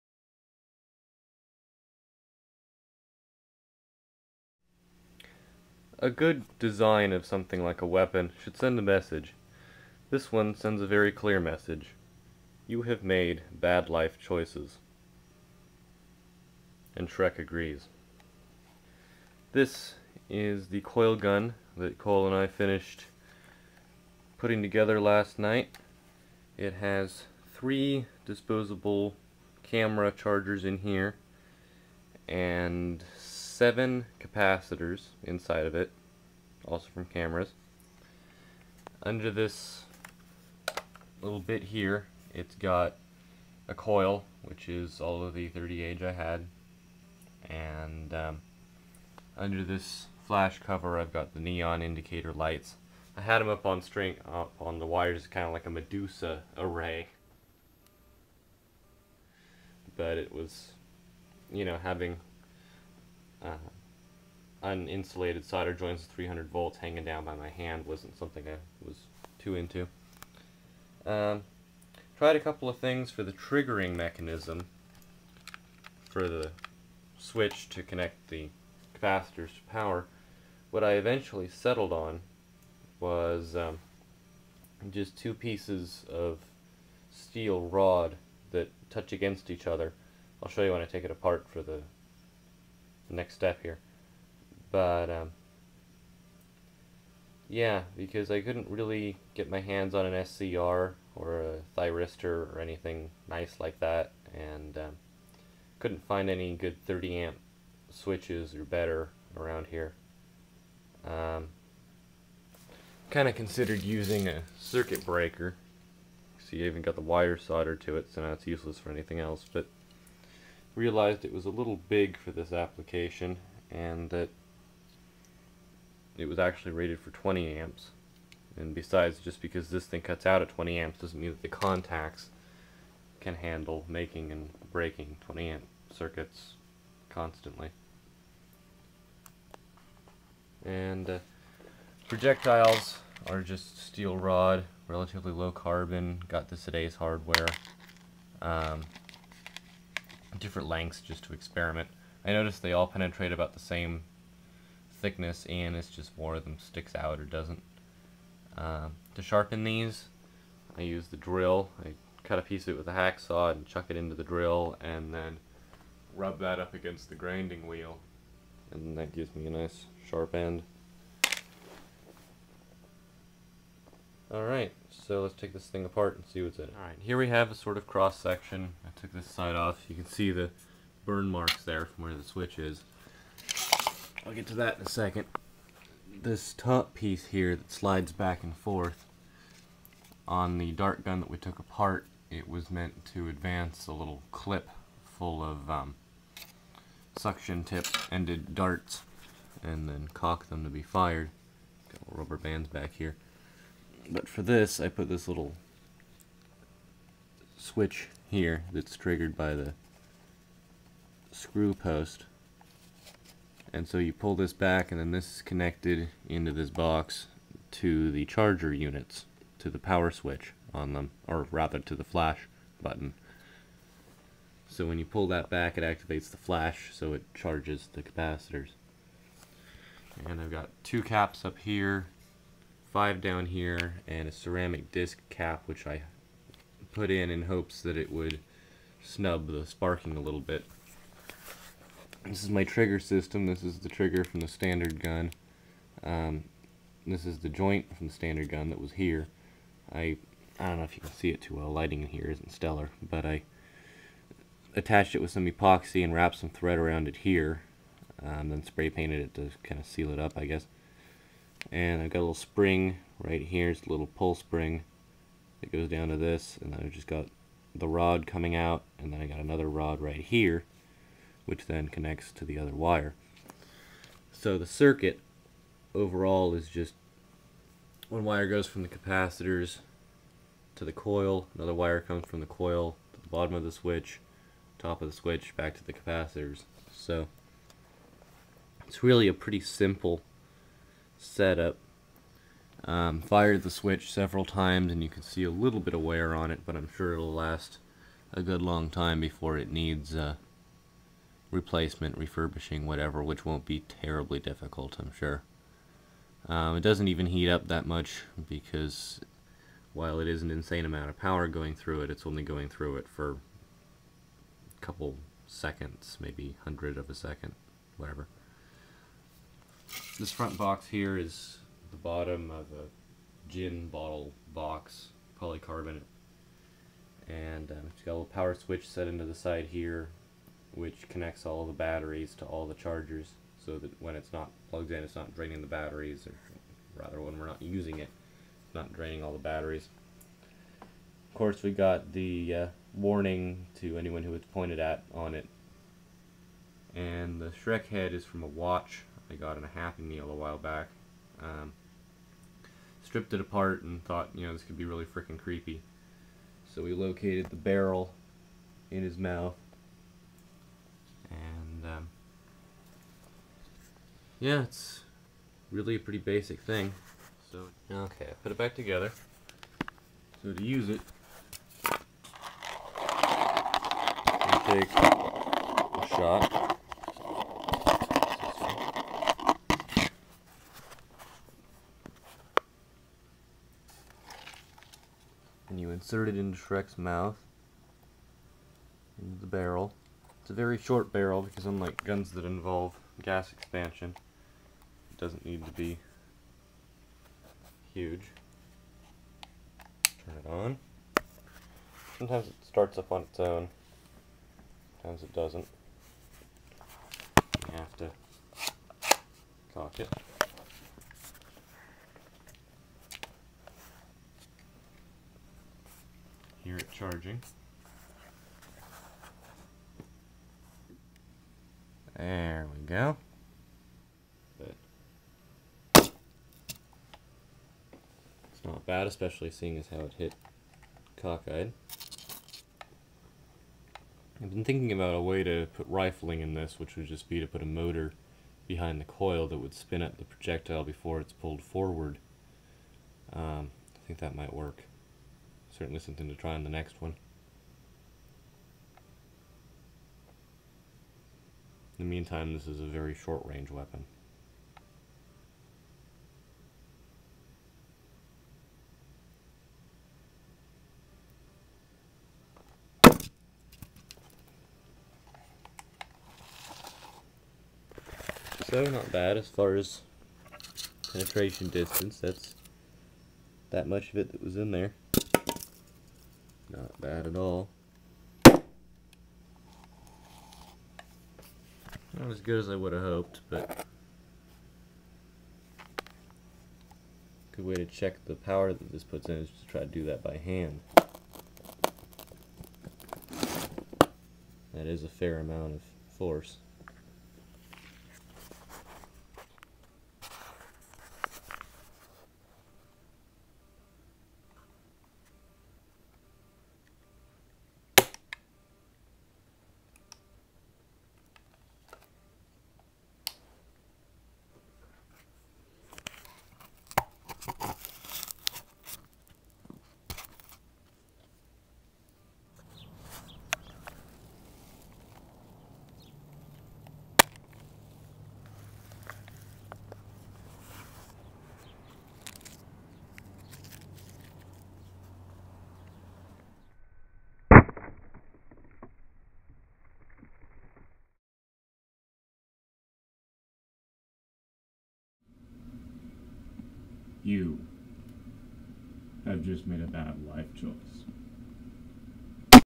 Coilgun is DONE
The coilgun has come together nicely. It's not the most powerful thing ever made but we're pretty happy with it.

Although, of course, we are already talking about the next model.